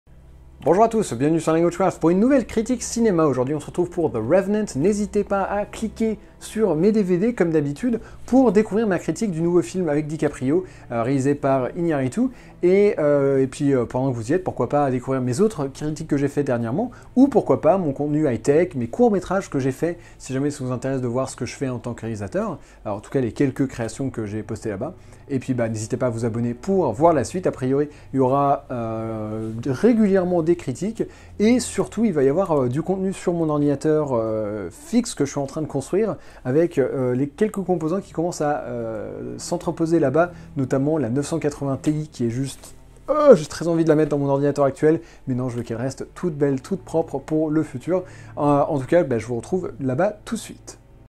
[FR] Impressions - The Revenant [Cinéma]
The Revenant raconte l'histoire de Hugh Glass, un trappeur qui se fait massacrer par un ours, et qui se bat malgré tout pour survivre !

Vidéo réalisée entre autres grâce au soutien de la communauté